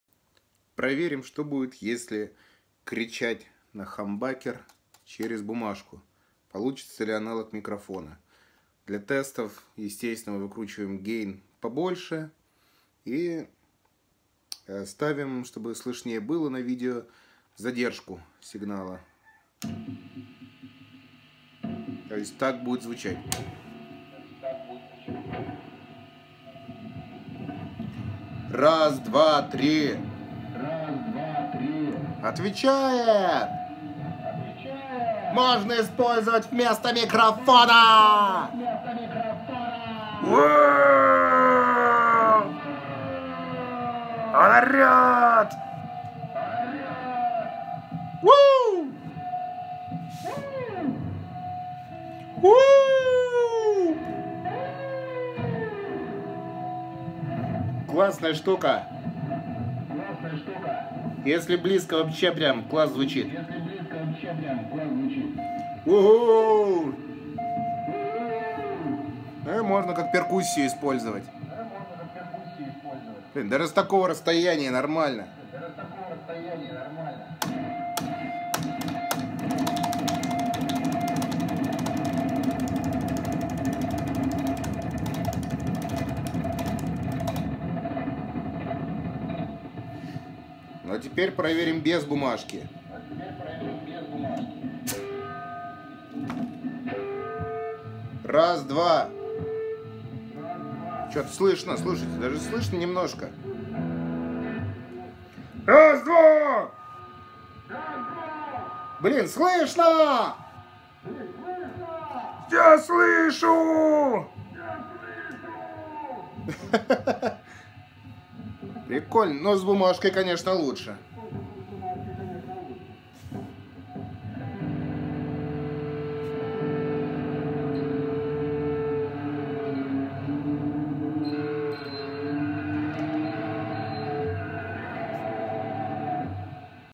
Можно ли использовать электрогитару как микрофон? Да!
Если вставить бумажку в струны над звукоснимателем, то она работает как мембрана микрофона: звук голоса передаёт вибрацию бумажке, а та передаёт вибрацию струнам. Звукосниматель это улавливает!

В конце я пробую и без бумажки. Это работает в моём случае, так как вибрирует пикгард вместе со звукоснимателями. Но звук выходит значительно тише, чем с бумажкой.